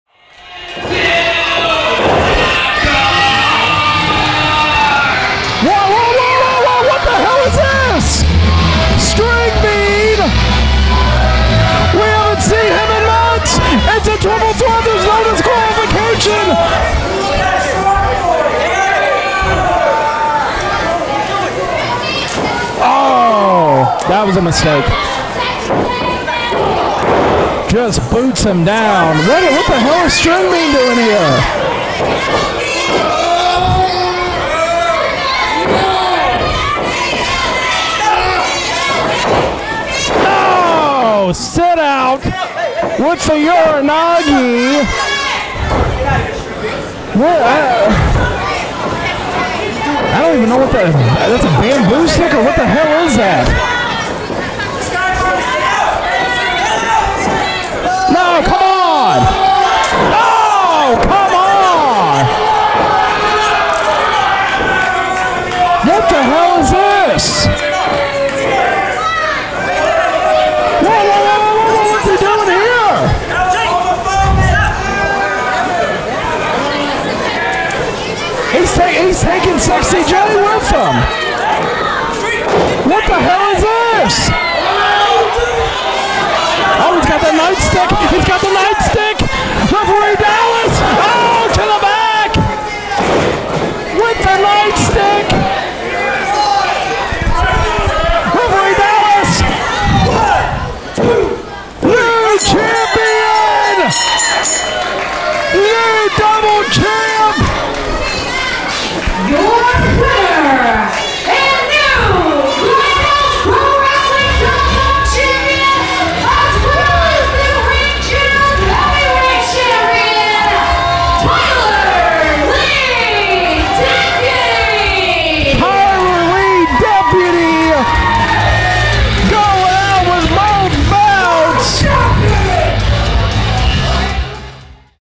And NEW DOUBLE Champion!
SO MUCH happened during the Triple Threat "Winner Take All" match at Summer Sweat Fest 2023 🌞💦🎉 but we have a NEW DOUBLE Champion!